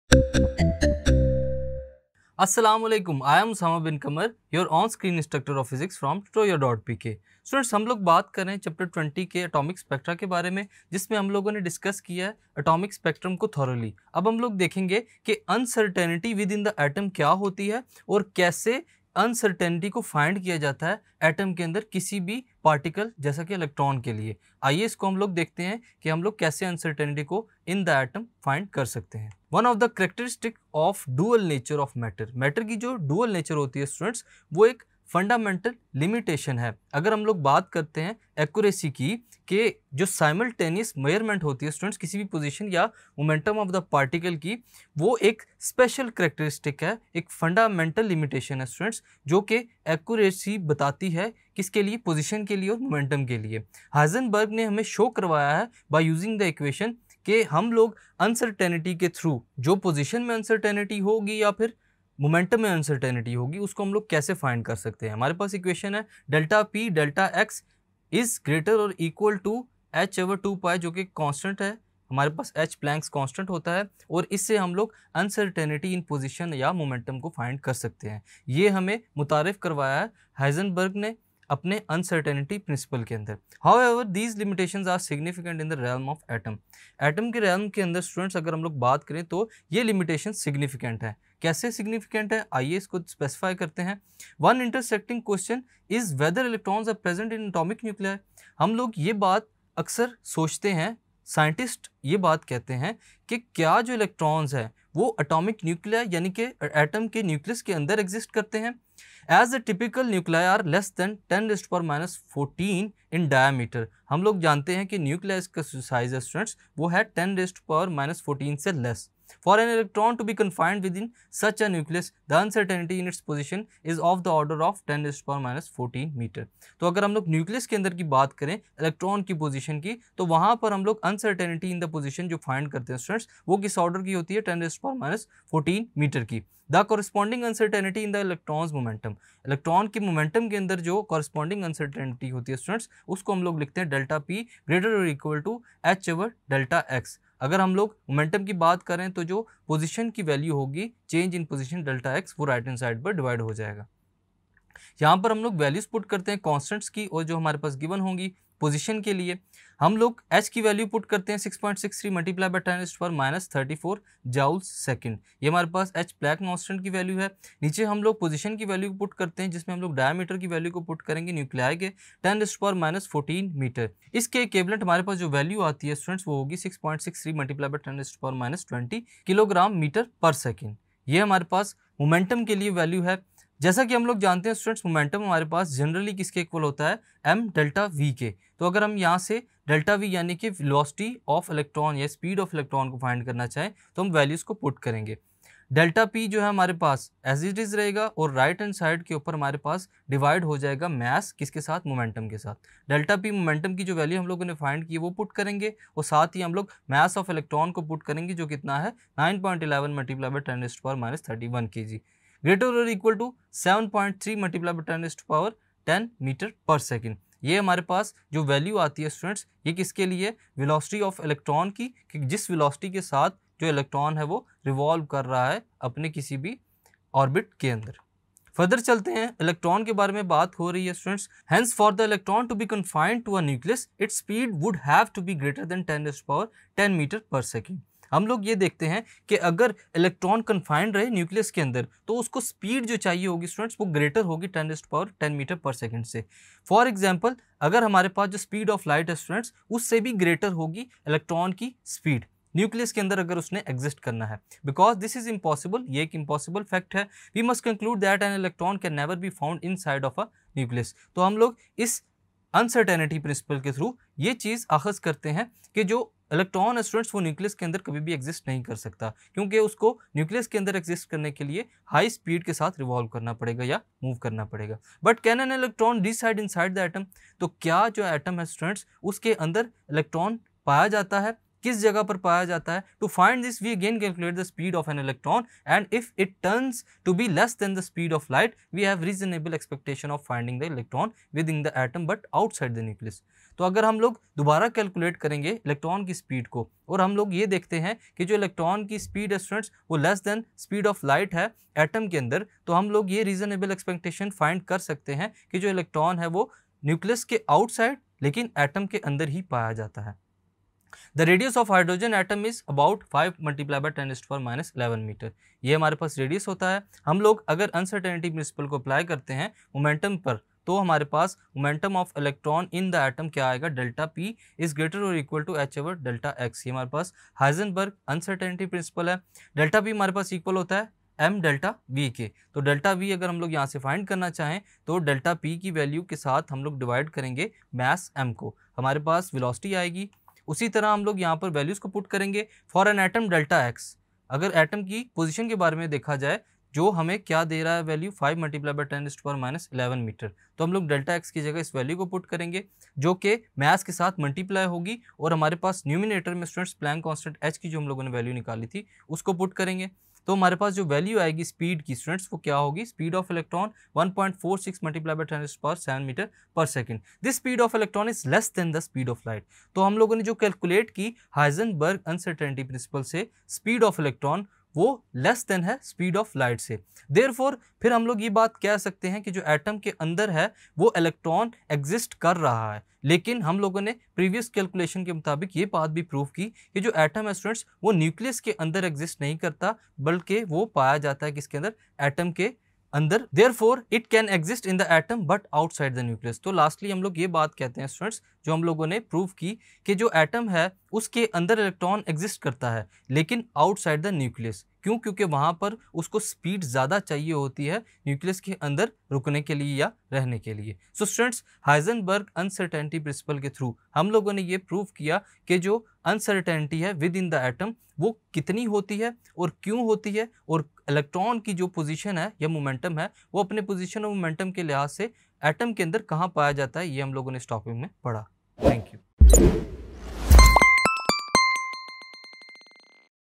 Physics Class 12 | Punjab Board | Ch 9 | Topic 2.1 | Uncertainty Within the Atom | tutoria.pk
Let’s take a look at the Chapter 9 of Physics of Grade 12 Punjab Board “Atomic Spectra”.

Learn about the atomic spectra and its fundamental properties. The lecture covers Bohr model of Hydrogen atom with Energy level diagram.

In this video, you will study Physics Grade 12 Punjab Board Chapter 9 Topic 1 "Uncertainty Within the Atom" by instructor Usama Bin Qamar.